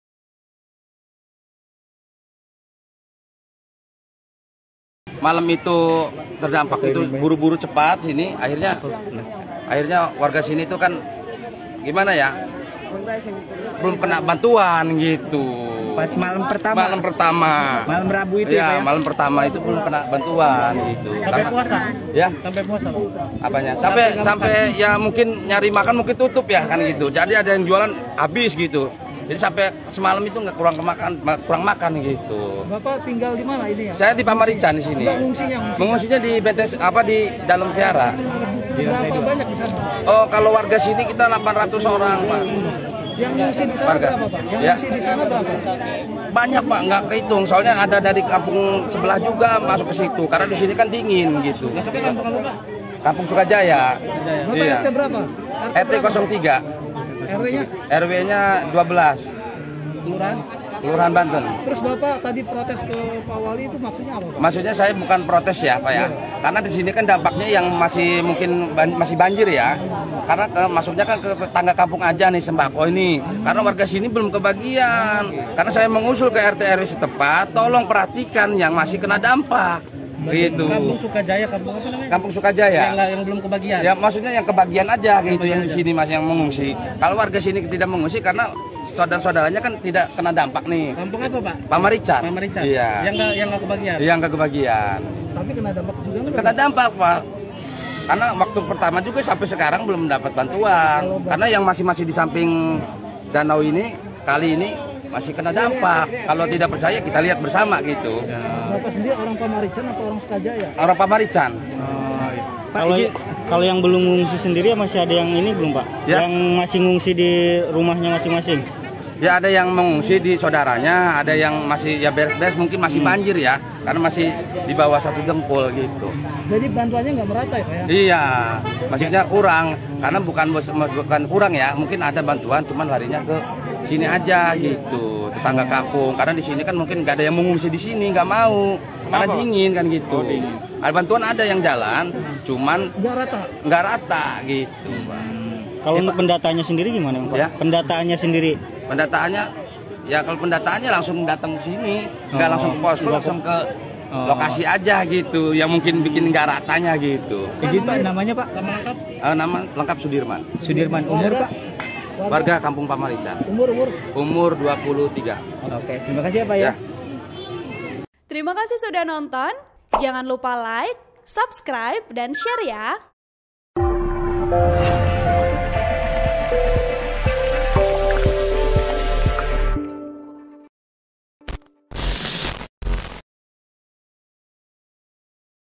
Curhat Warga Korban Banjir ke Wali Kota Serang Cari Makan Susah, Tolong
TRIBUN-VIDEO.COM - Sejumlah warga Kota Serang protes soal bantuan bagi korban bencana banjir kepada Wali Kota Serang, Syafrudin.

Sudirman, seorang warga Pamarican RT/RW 03/12 Kelurahan Banten, Kecamatan Kasemen mengaku belum mendapat bantuan selama tinggal di pengungsian.

"Kami tinggal di pengungsian di Vihara, sampai saat ini belum pernah mendapat bantuan," ujarnya kepada awak media saat di lokasi, Kamis (3/3/2022).(*)